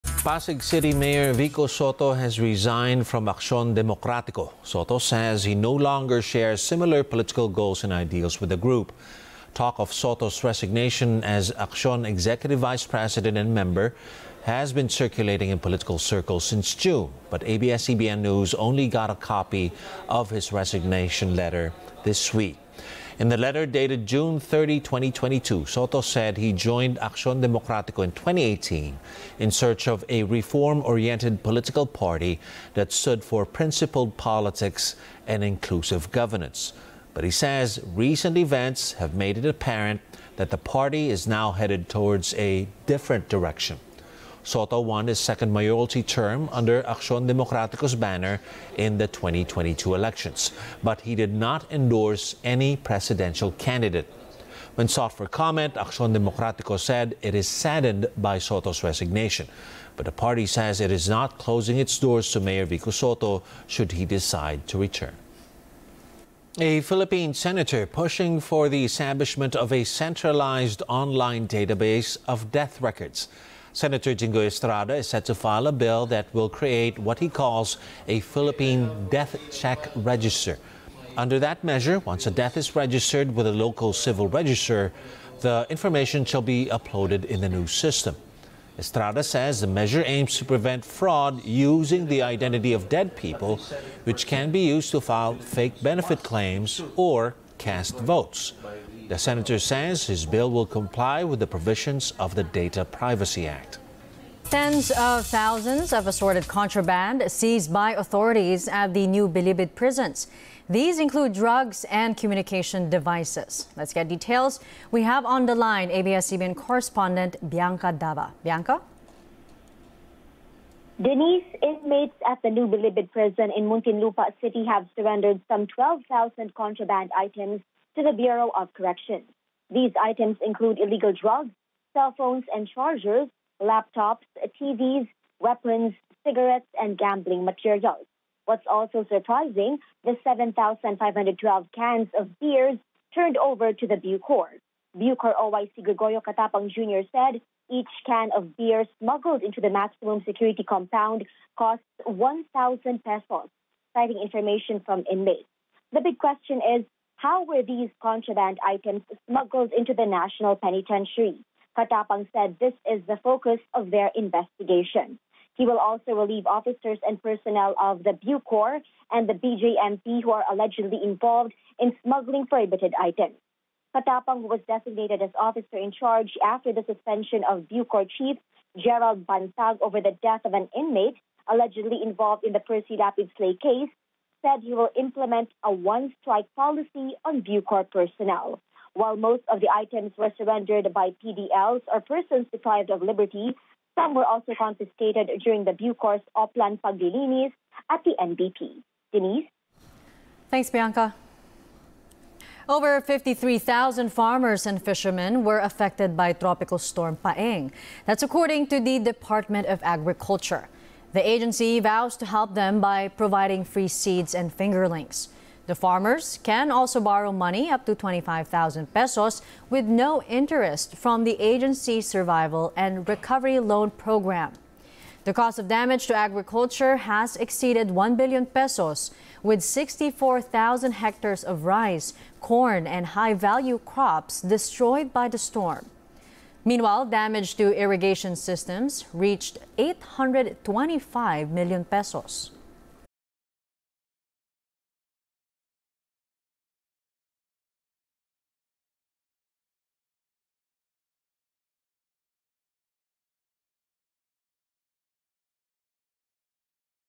Vico Sotto no longer with Aksyon Demokratiko due to difference in ‘political goals, ideals’ | ANC
Top Story: Pasig city Mayor Vico Sotto has resigned from Aksyon Demokratiko.